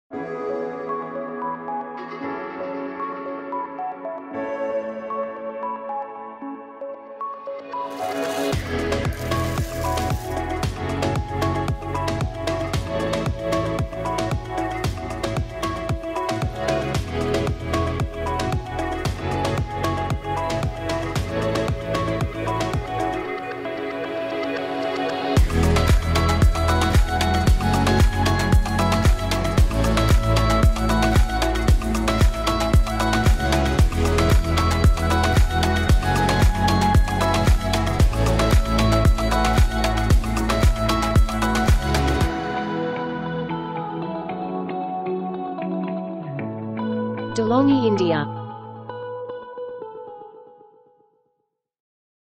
DELONGHI DINAMICA PLUS FEATURES REVIEW
DELONGHI COFFEE MACHINE SELLER/SHOWROOM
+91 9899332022 Call for Best Price

De'Longhi Dinamica Plus ECAM370.85.SB with 3,5” full touch TFT color display, 4 soft touch buttons, size and aroma selection, milk carafe My function, Pot function, coffee link app, silver

Makes all coffee shop favourites, grinding your choice of fresh beans for a perfect taste. Makes up to two cups in a single brew
Digital display mith My Milk function provides easy menu navigation and drinks personalisation: Programme the strength, length and temperature to your taste
Painted finishing and colour touch interface
Create new recipes, customize aroma and quantity, manage profiles via display & Coffee Link App
Lattecrema automatic milk frothing carafe serves cappuccinos, lattes, flat whites and more straignt to your cup. With autoclean: return to fridge after use